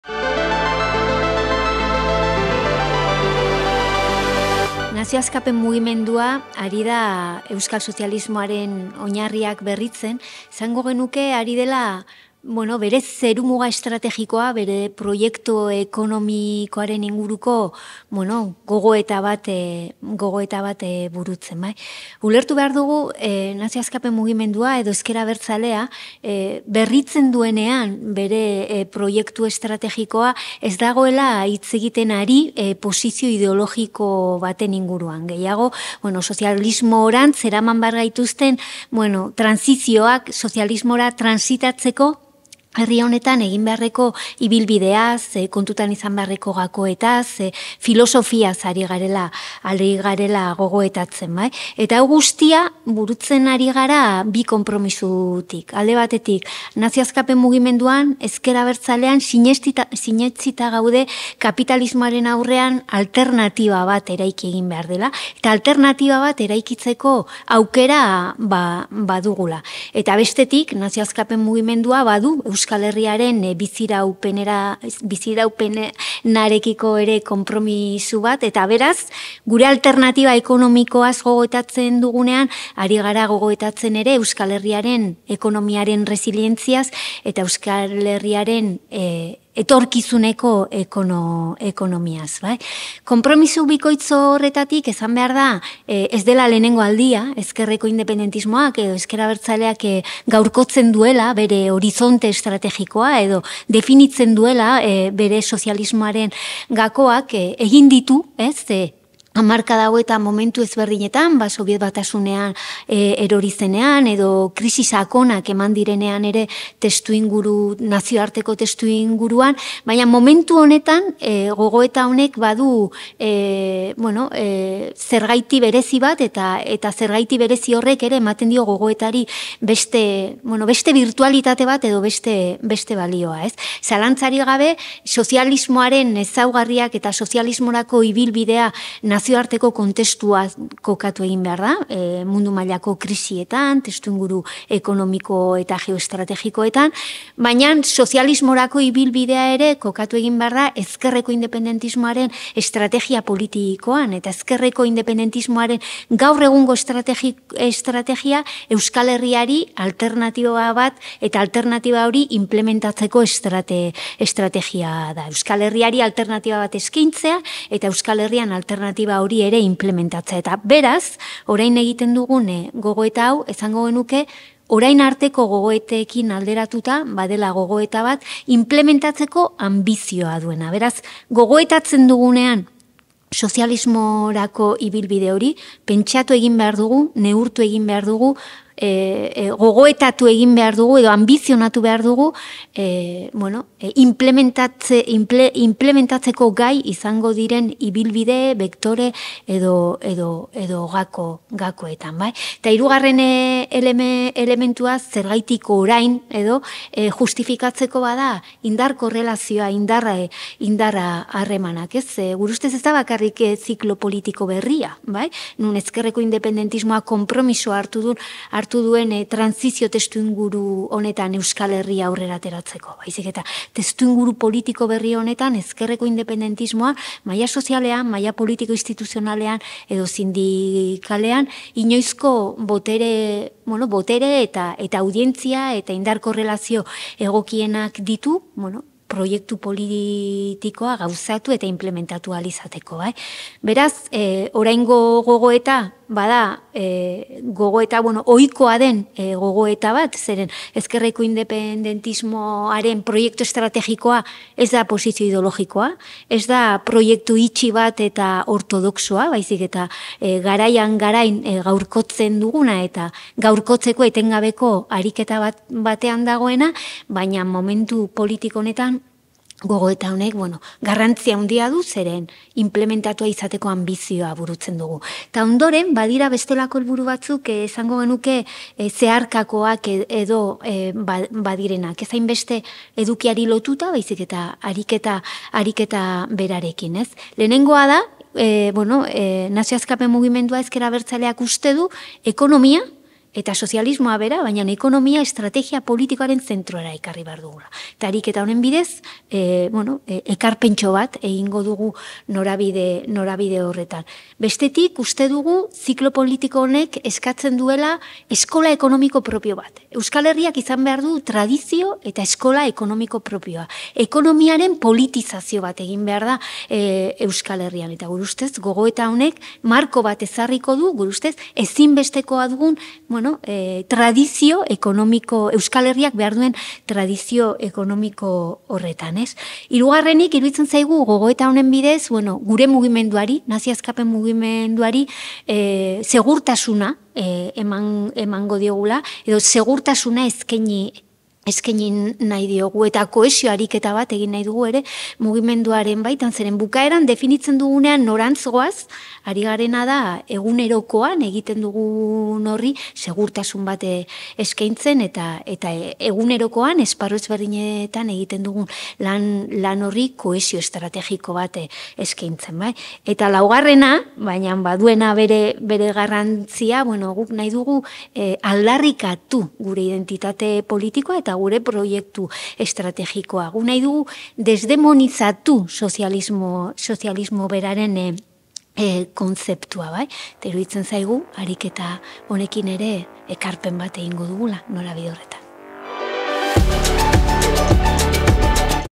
Elkarrizketa | KIZKITZA GIL DE SAN VICENTE Euskal sozialismo berriaren gogoeta
Euskal sozialismoaren oinarriak berritzeko hausnarketa egiten ari da ezker abertzalea. Zertan datza?